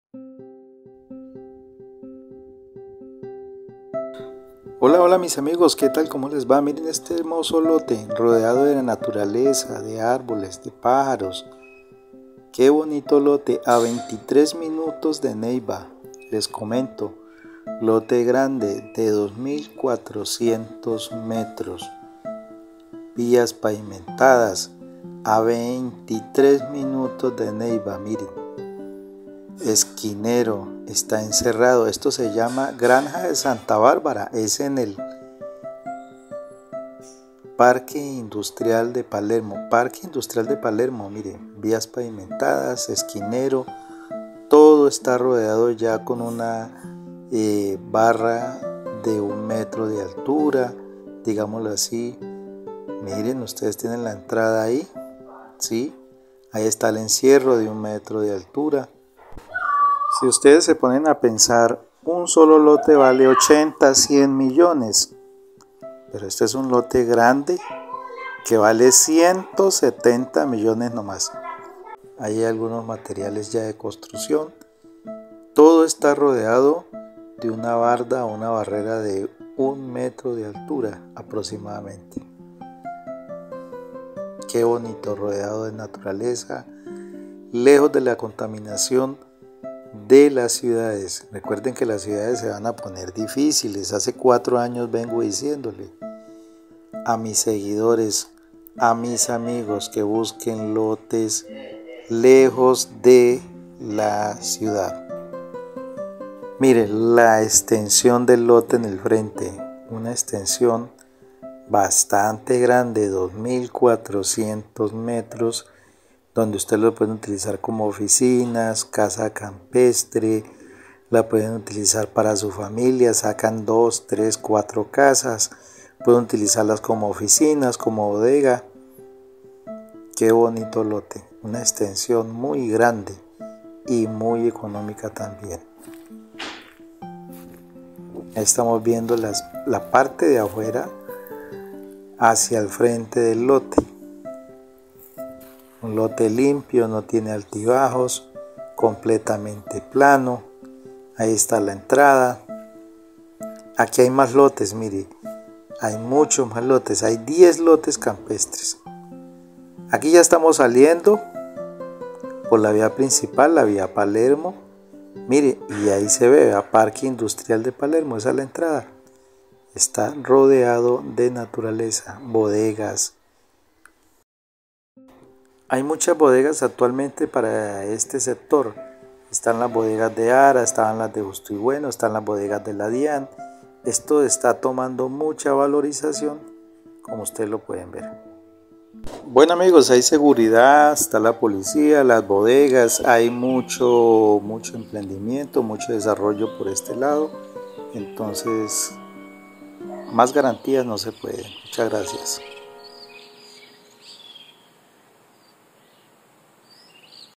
Un gran lote en venta con buenas energías y con escrituras ❤️ Gabriel Gutierrez
En este lote prosperará todo lo que hagas y lo que construyas!